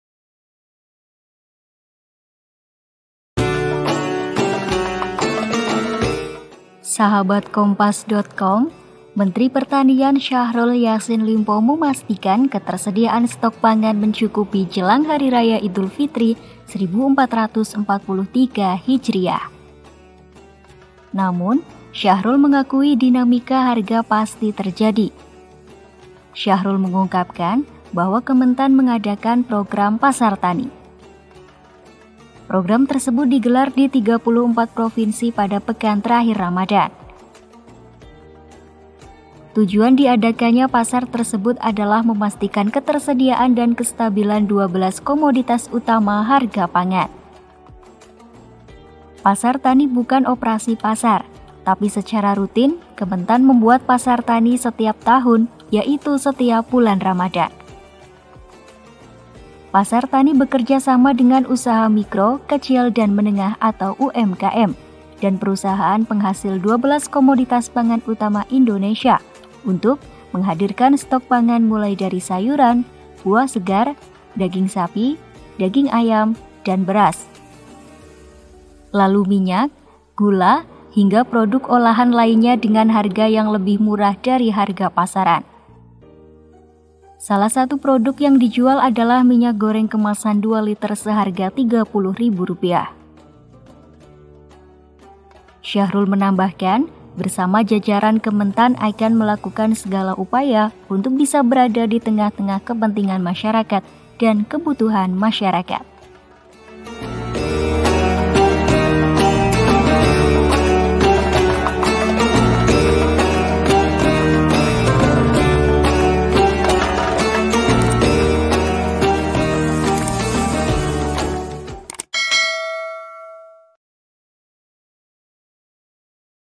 Menteri Pertanian Pastikan Stok Pangan Aman Jelang Idul Fitri
Menteri Pertanian (Mentan) Syahrul Yasin Limpo memastikan ketersedian stok pangan mencukupi jelang Hari Raya Idul Fitri 1443 Hijriah. Syahrul mengungkapkan bahwa Kementan mengadakan program Pasar Tani. Program ini digelar di 34 provinsi pada pekan terakhir Ramadhan. Tujuan diadakannya pasar ini adalah memastikan ketersediaan dan kestabilan 12 komoditas utama harga pangan.
Pasar Tani bekerja sama dengan usaha mikro, kecil, dan menengah (UMKM) dan perusahaan penghasil 12 komoditas pangan utama Indonesia untuk menghadirkan stok pangan mulai dari sayuran, buah segar, daging sapi, daging ayam, beras, minyak, gula, hingga produk olahan lainnya dengan harga yang lebih murah dari harga pasaran.
Video Jurnalis: KompasTV/I Wayan Hobiana, Prahayuda Febrianto, Fadlyanto Sugiono, Arpawi,
Penulis: Dian Erika Nugraheny
Penulis Naskah: Arini Kusuma Jati
Video Editor: Menika Ambar Sari
Produser: Agung Wisnugroho